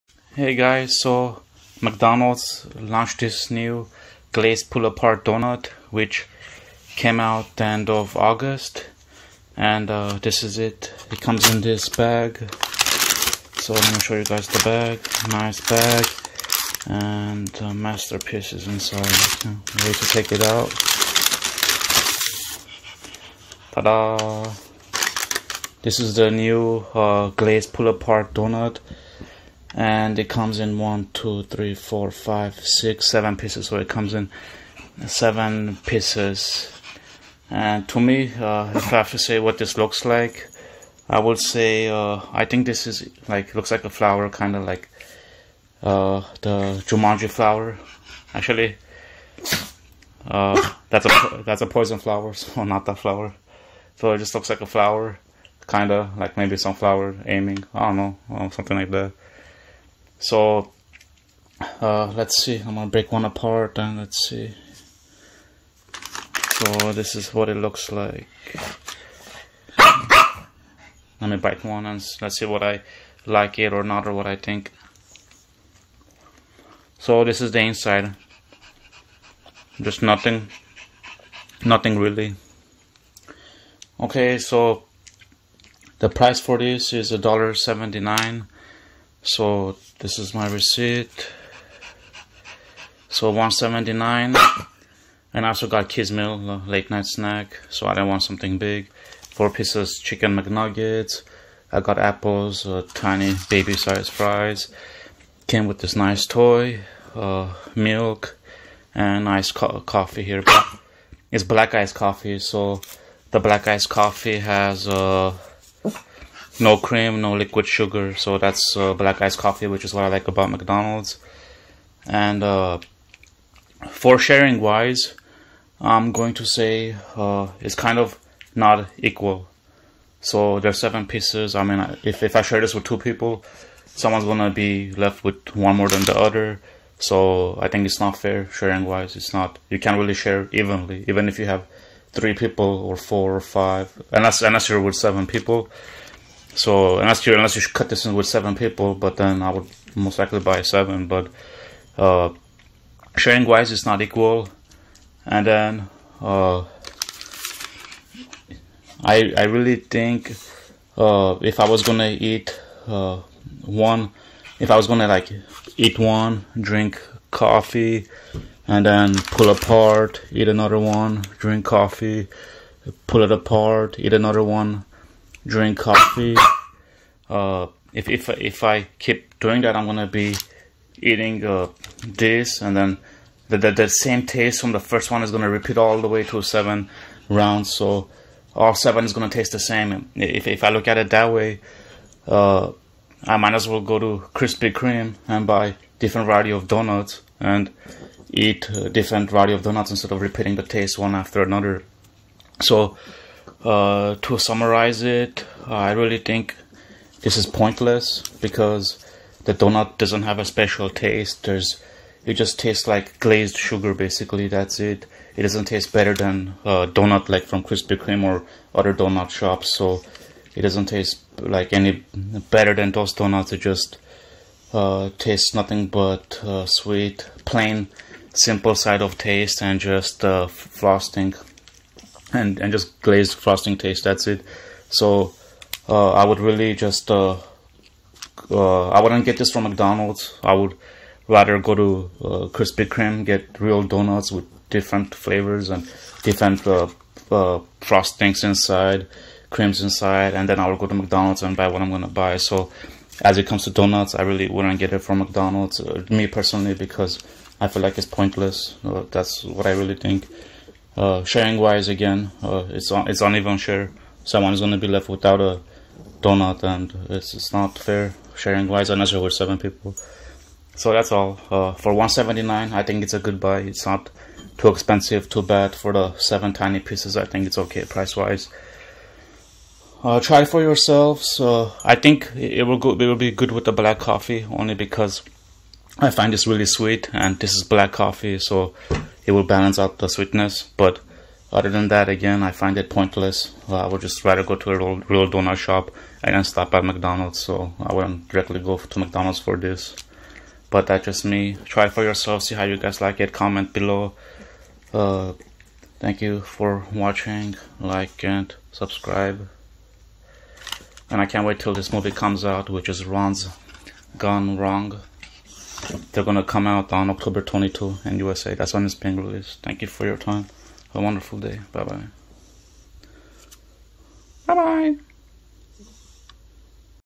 McDonald's Glazed Pull Apart Donut Review
This is the limited time McDonald's Glazed Pull Apart Donut and comes in 7 pull apart pieces. I find it not fair in terms of sharing because it doesn't have even number of pieces which means someone's going to have 1 donut more than the other.
It doesn't have anything special in taste and nothing unique, just a simple glazed sugar donut and that's all, very basic.
I really think its pointless because I would just stop at a real donut shop and buy variety of different flavor donuts and then I'll go to McDonald's for my meal. By having variety flavored donuts I will enjoy more and it will be less boring in taste.